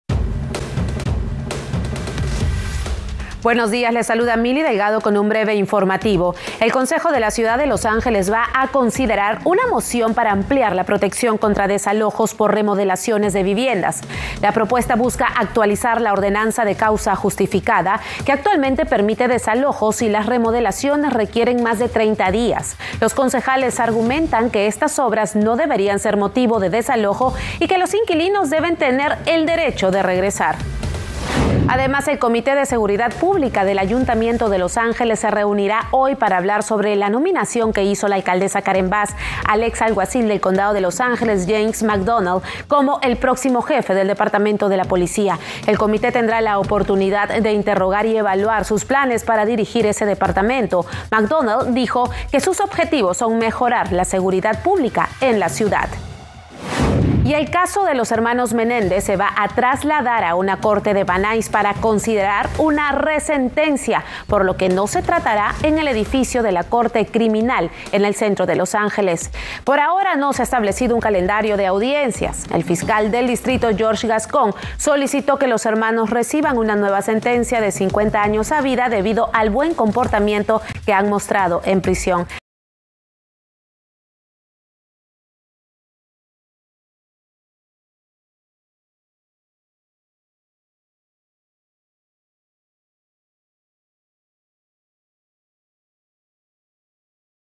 Considerarán moción sobre desalojos en LA: las noticias más importantes del día en dos minutos
El Concejo de Los Ángeles considerará una moción para ampliar la protección contra desalojos por remodelaciones de viviendas, buscando actualizar la ordenanza de causa justificada. En otras noticias del día, el comité de seguridad pública del ayuntamiento de la ciudad se referirá a la nominación de Jim McDonnell como el próximo jefe de LAPD, y el caso de los hermanos Menéndez se trasladará a una corte de Van Nuys.